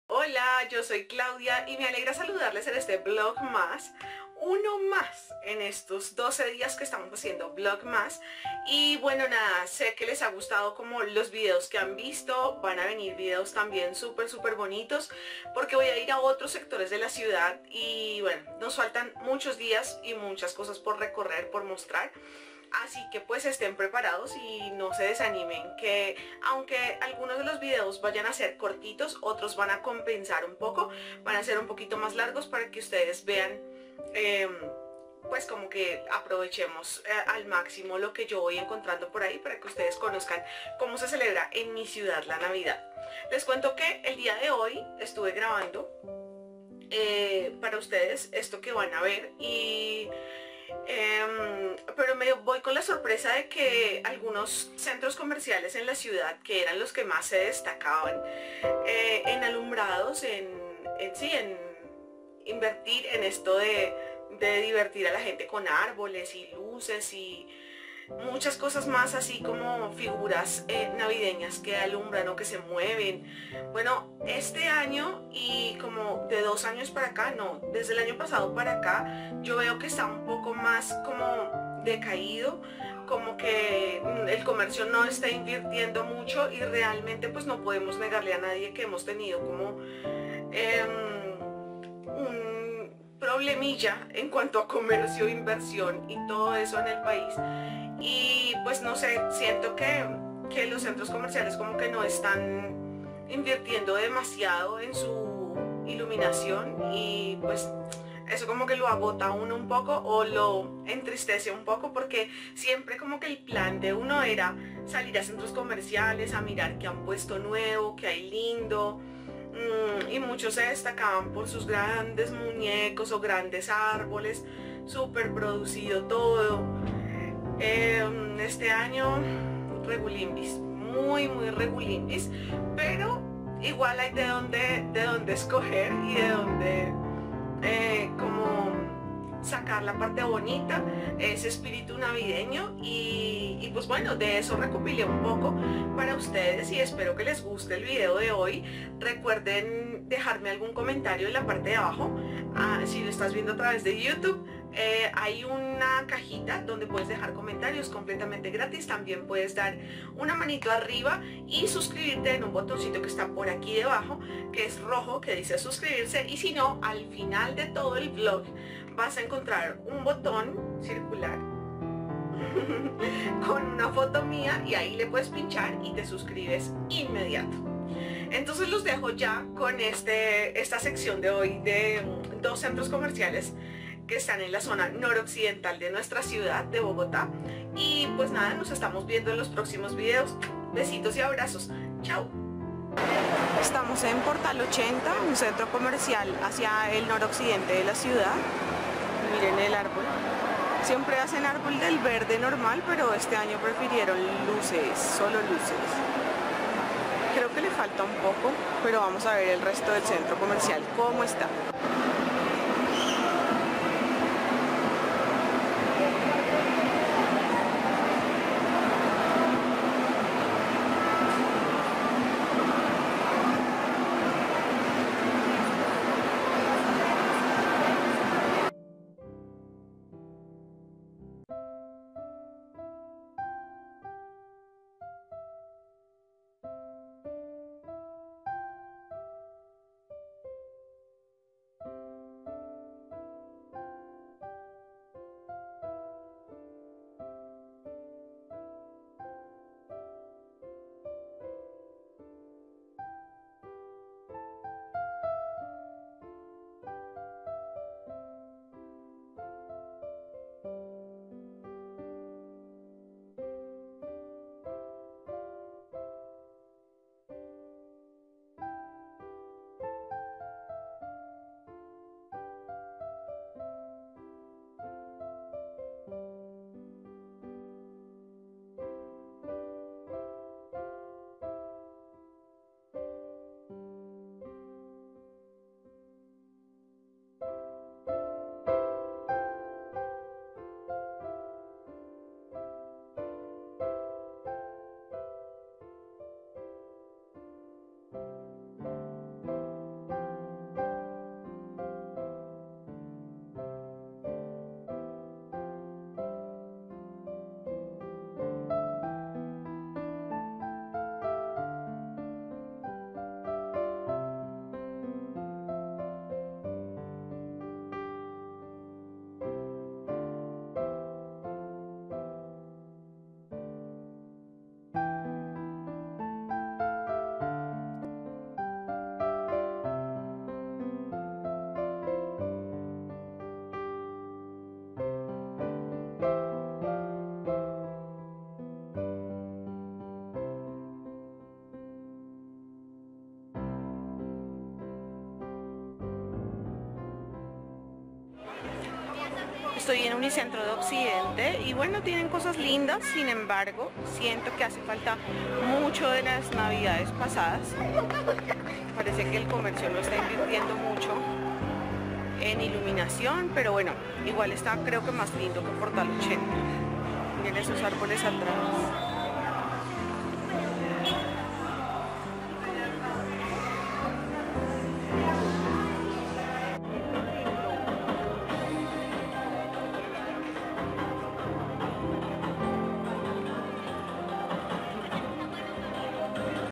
VlogMas 4 Centros Comerciales
VlogMasConXimena VlogMas2016 Esta serie de videos donde te comparto la Navidad en mi ciudad.
Acompañando al grupo de Ximenadelaserna.com

Claumelocoton

Periscope
Claumelocoton